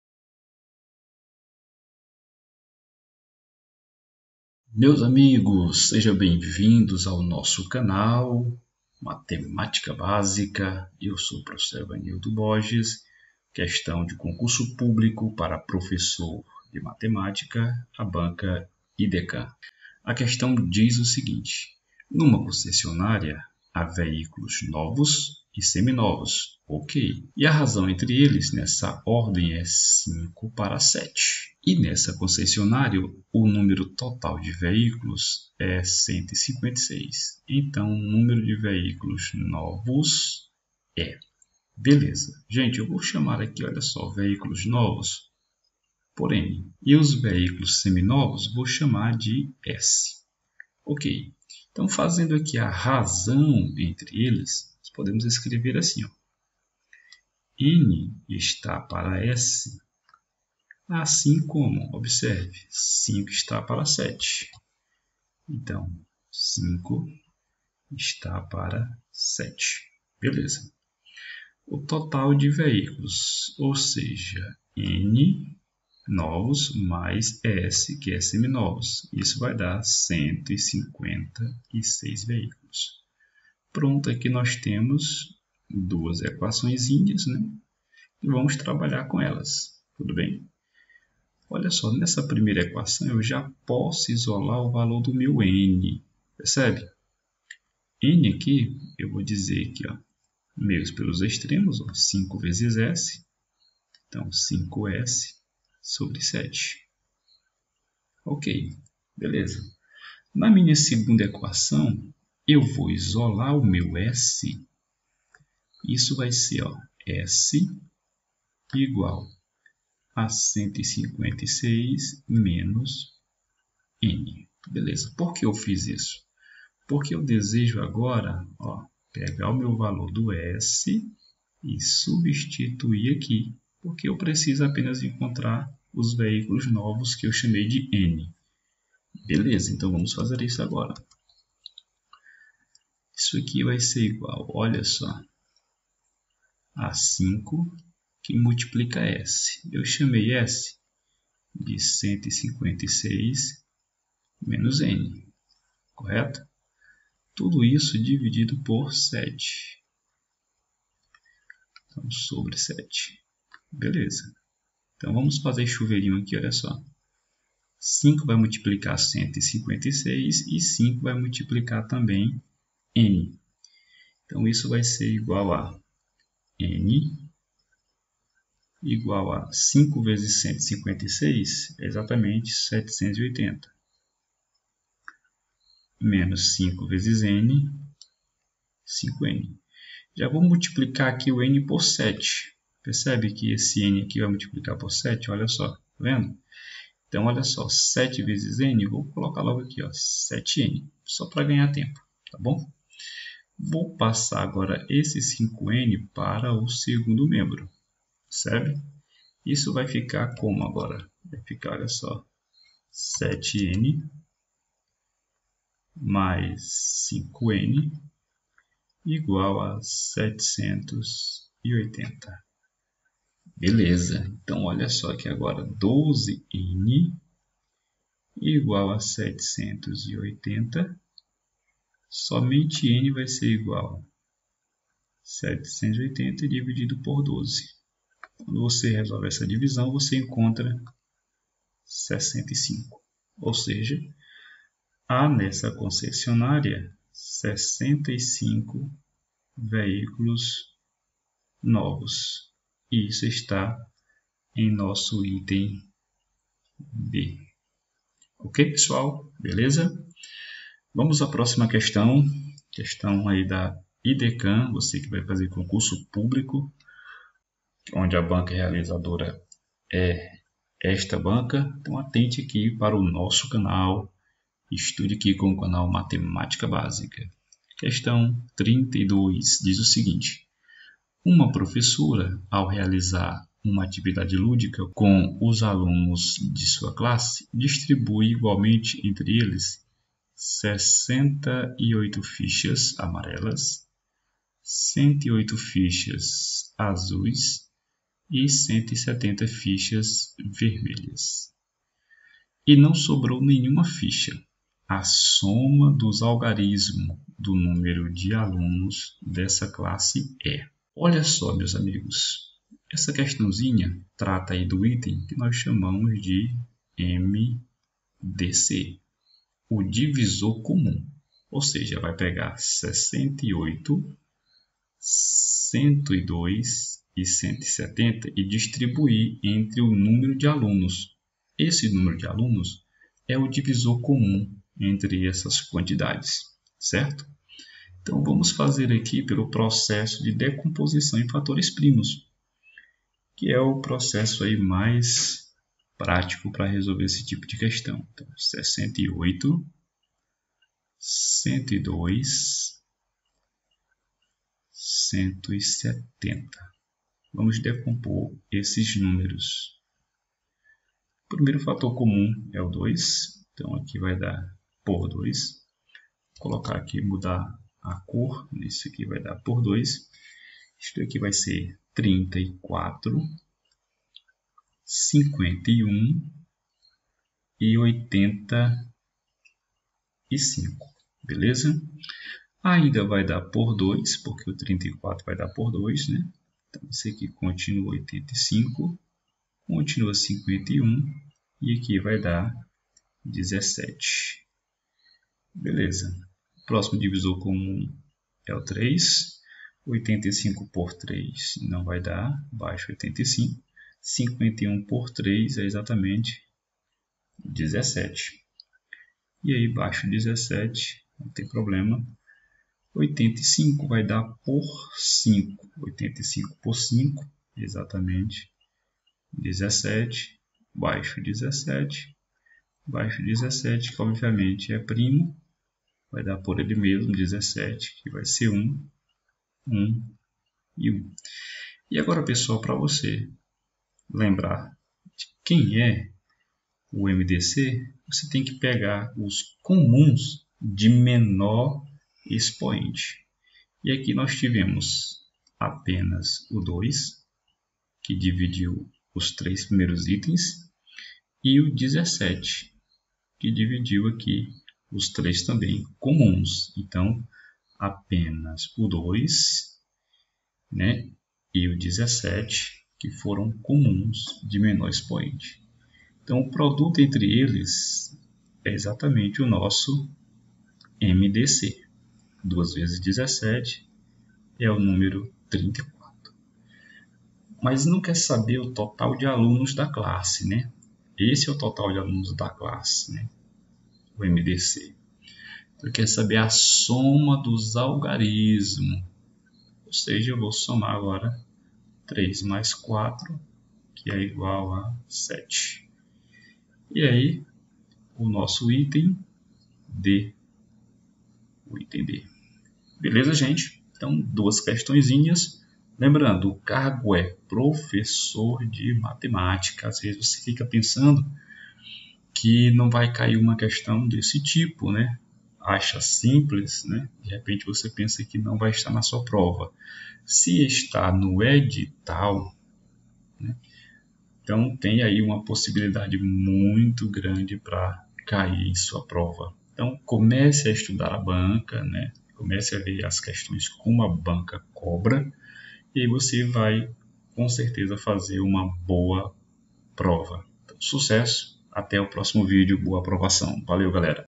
V1 - Questões 31 e 32 - Equações e MDC - Cargo: Prof de Matemática - Banca: IDECAN
Pessoal, bom dia!!!
Estou iniciando as gravações de questões de concurso público realizado por esta banca, a IDECAN.
Neste vídeo temos duas questões de Matemática, para o cargo de Prof de Matemática, do concurso público municipal de Manhumirim - MG.